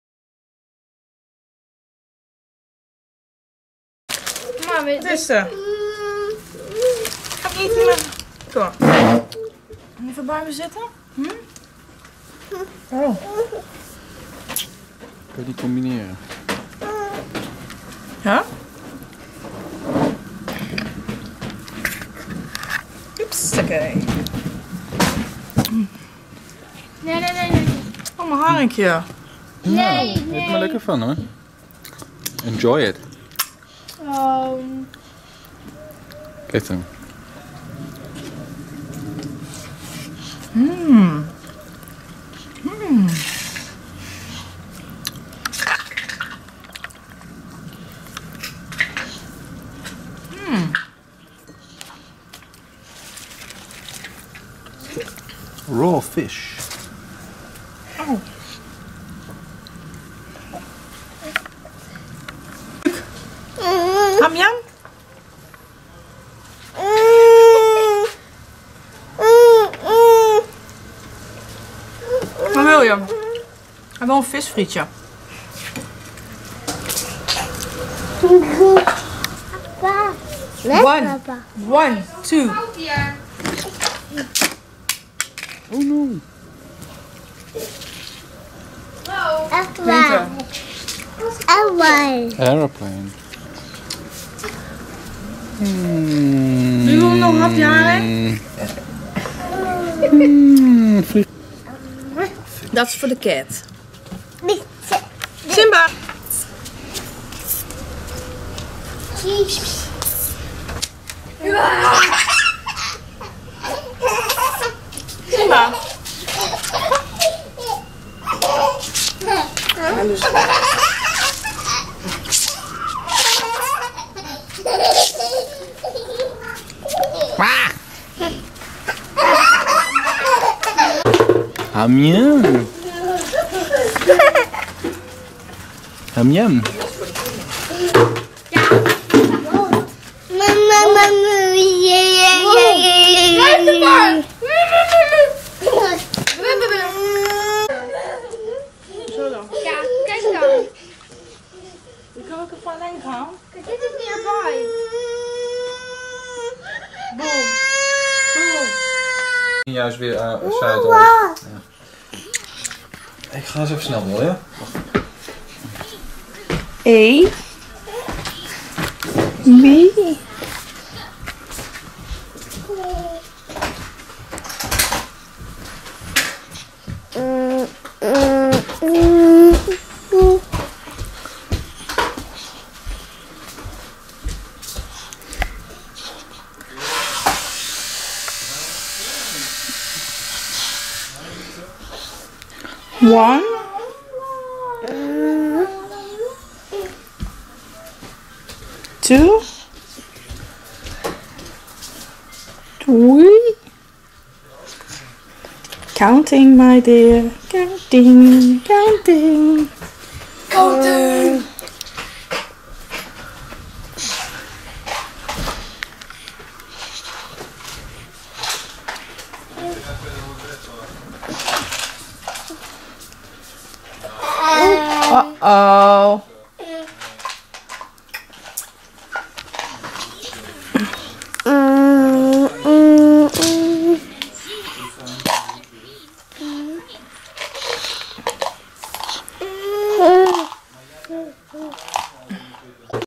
BREASTFEEDING EATING RAW FISH/ Learning
One of the twin can't what breastfeeding while i am eating RAW fish. They are so lovely xxx